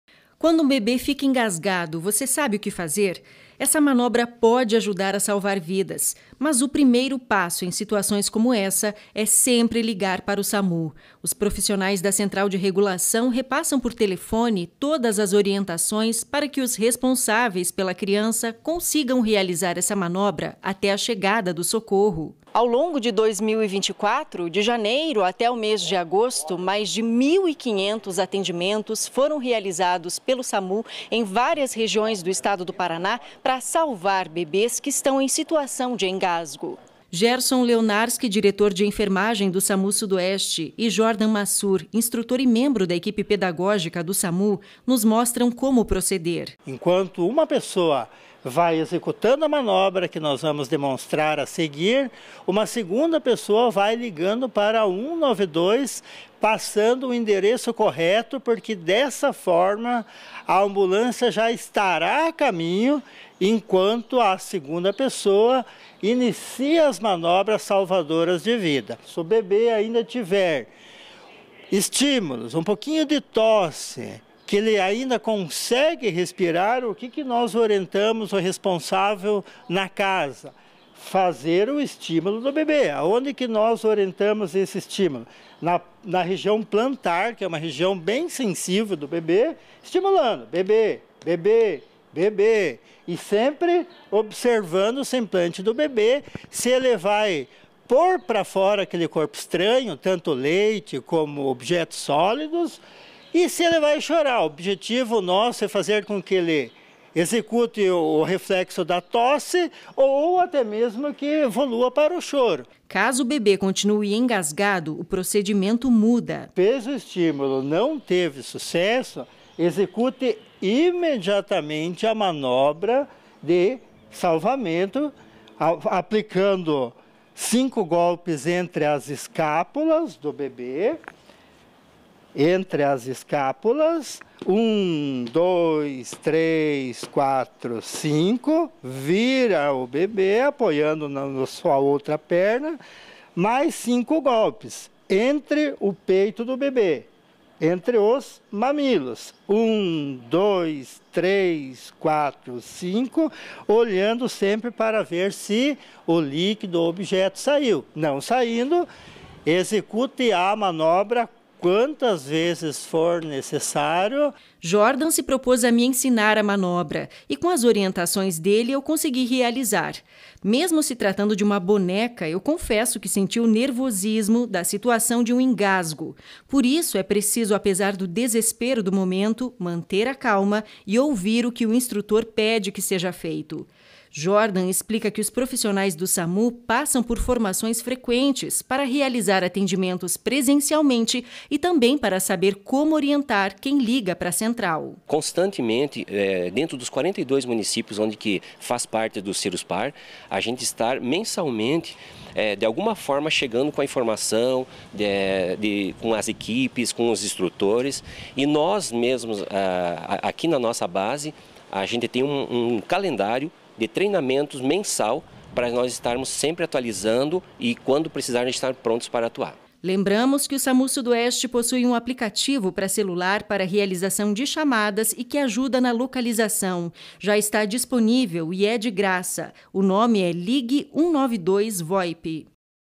SAMU ensina manobra de desengasgo em bebes
Segurança: Somente este ano o SAMU atendeu mais de 1.500 casos de engasgo. O maior número de ocorrências é entre bebes de até um ano de idade. Nossa equipe entrevistou profissionais do SAMU sudoeste, sobre como agir em situações como essa.